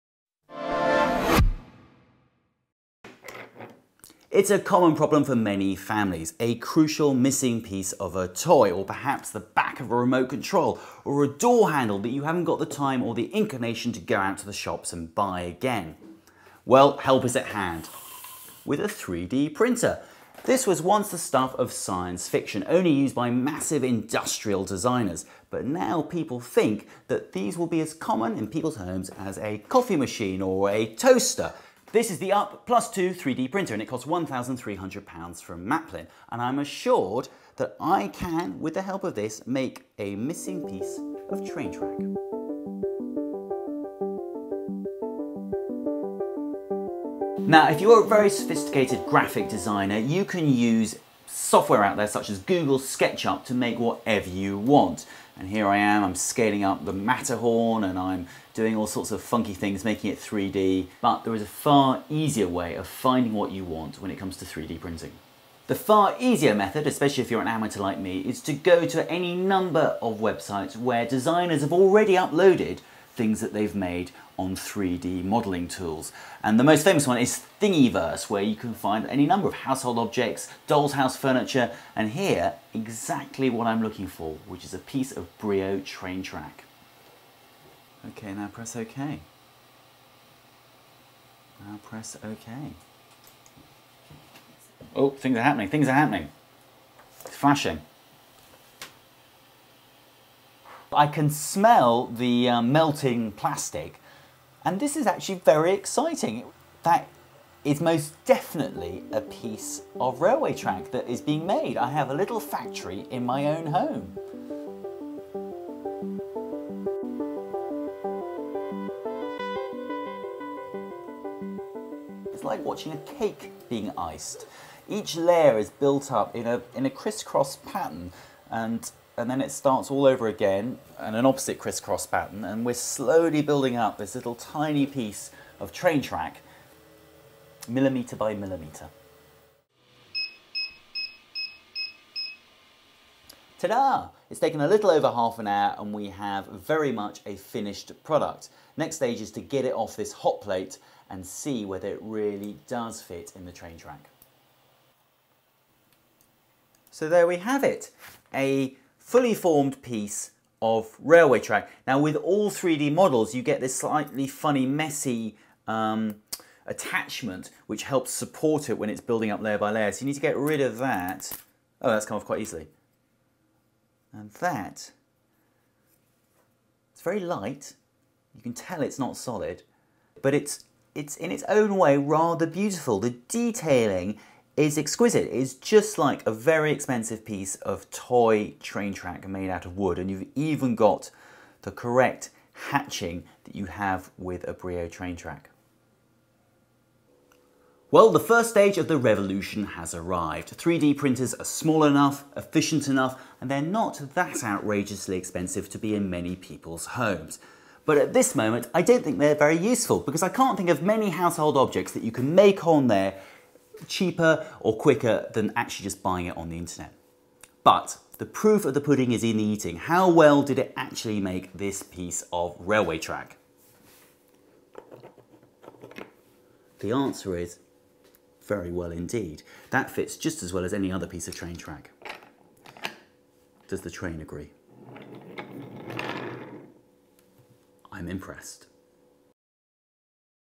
3D printer put through its paces
3D printers have produced guns, space food, and even human body parts but the Telegraph's Harry Wallop had a more modest aim -- to create a vital missing piece of track for his son's train set.

Although 3D printing has been widely used by design and manufacturing industries for many years the technology has only relatively recently been modified for a consumer market.

The Telegraph's Harry Wallop decided to test out the capabilities of the pp3dp printer which has just been released for use at home and retails at £1,300.00.

Harry's object of choice was the vital missing piece of a brio train track from his son's train set, he described the printing process saying:

 "Each layer is built up in a criss-cross pattern and then it starts all over again in an opposite criss-cross pattern and we're slowly building up this tiny piece of train track millimetre by millimetre."

The PPDP 3D printer claims to provide an "easier printing experience" through automatic features such as automatic nozzle calibration height.

The device melts ABS plastic to build 3D models layer by layer.